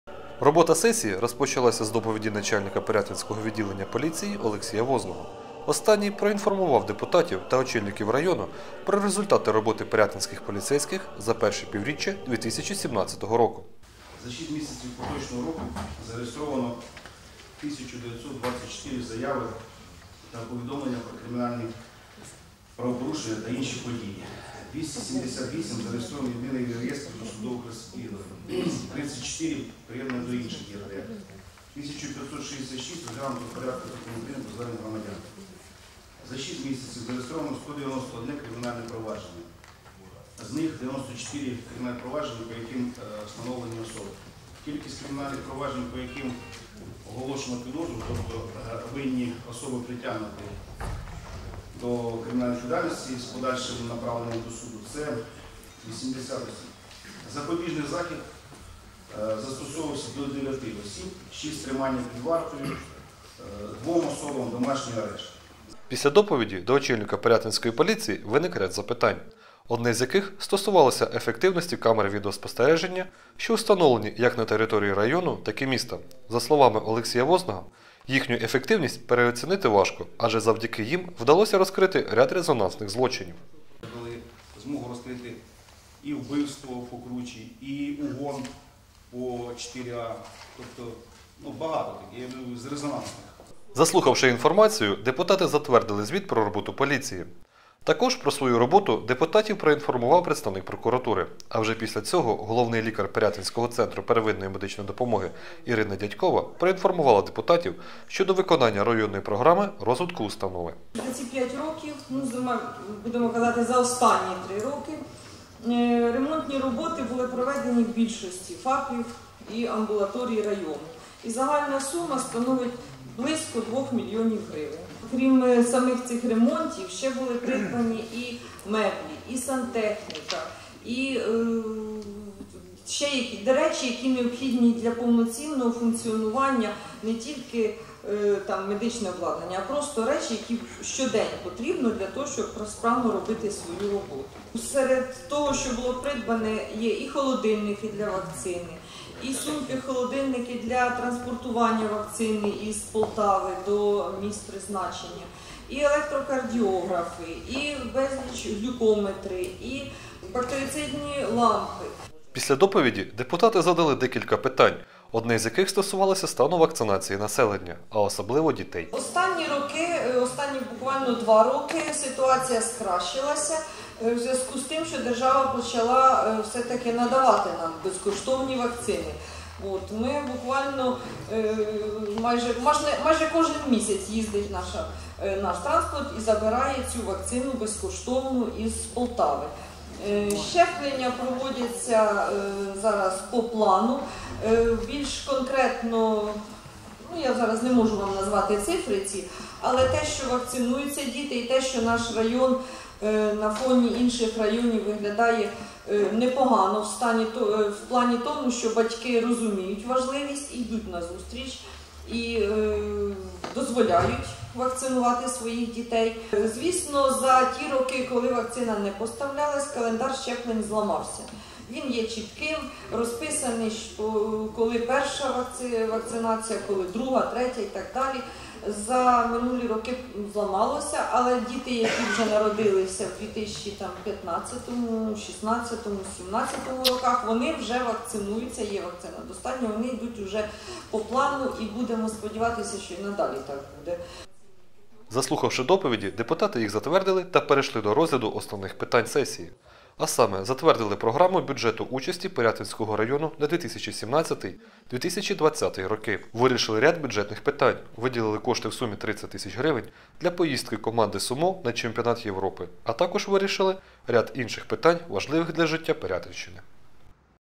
23 - тя сесія Пирятинської районної ради
17 жовтня у великій залі адміністративного будинку відбулася 23 - тя сесія Пирятинської районної ради. Які рішення приймали депутати знають наші журналісти.

Телерадіокомпанія "Пирятин"